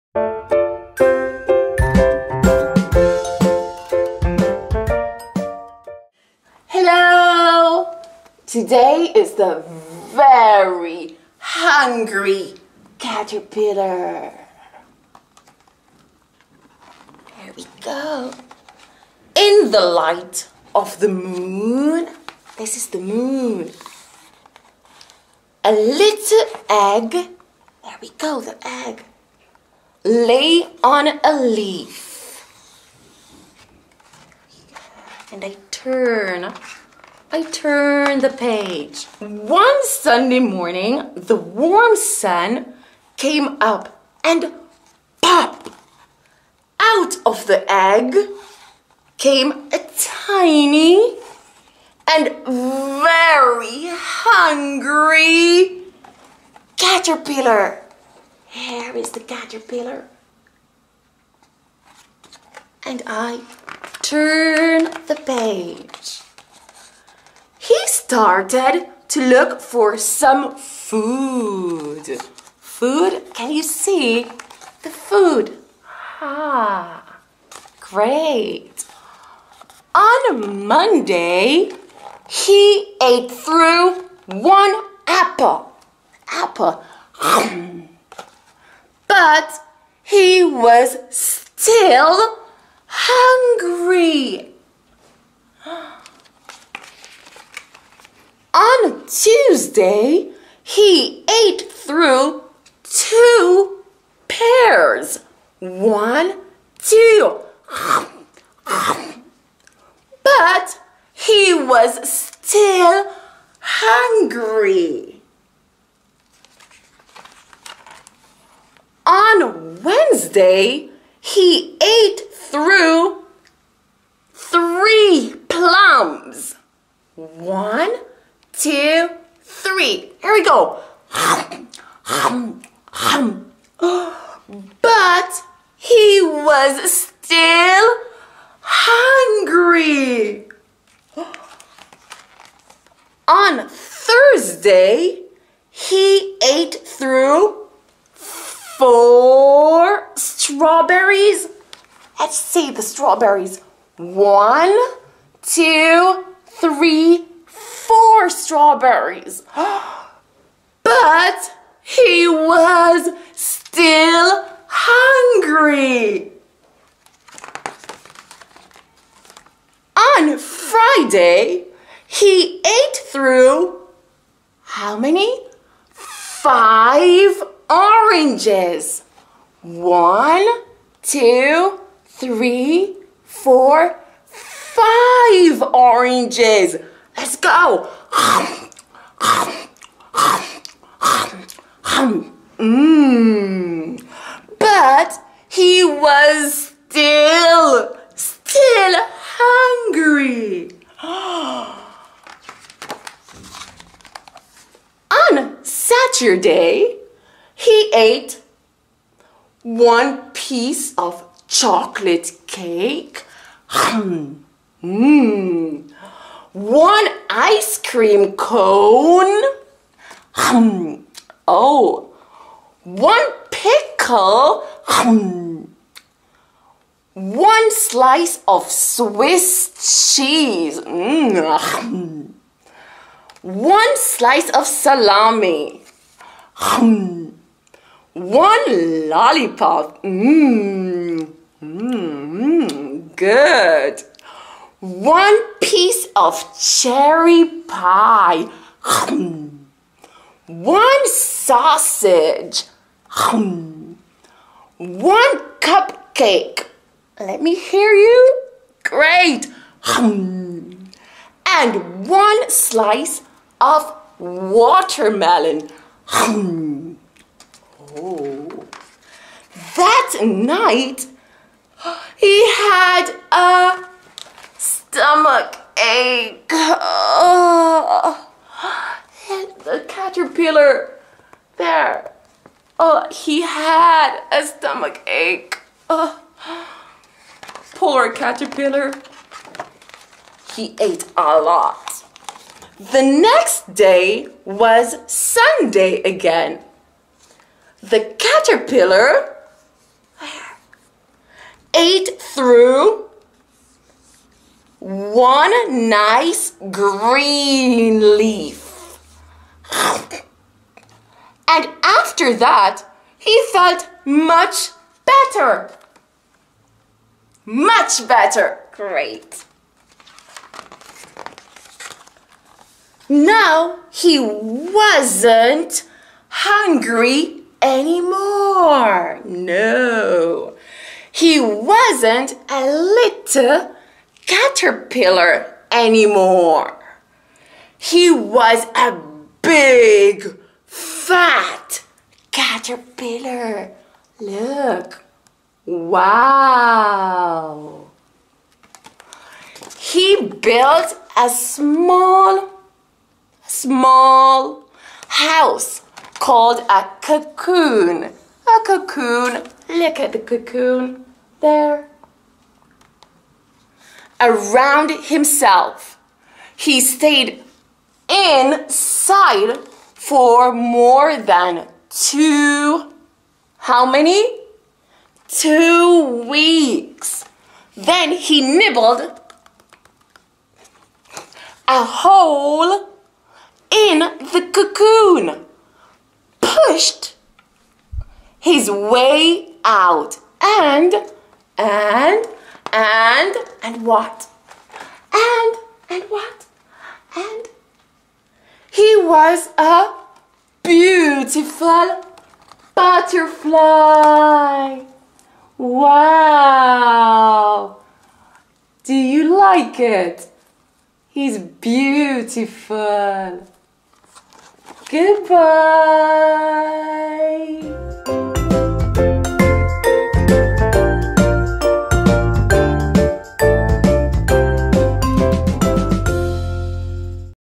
The Very Hungry Caterpillar - Linguish Story Telling avec le Furet du Nord!
Cette lecture de ce classique d'Eric Carle est réalisé avec la participation de "English Books by le Furet" par le Furet du Nord.
Vous cherchez des livres pour enfants en anglais? Retrouvez un grand choix de titre dans la section jeunesse! "The Very Hungry Caterpillar" est très fun et permet de jouer avec l'histoire.
Merci à Alicea notre super teacher! :D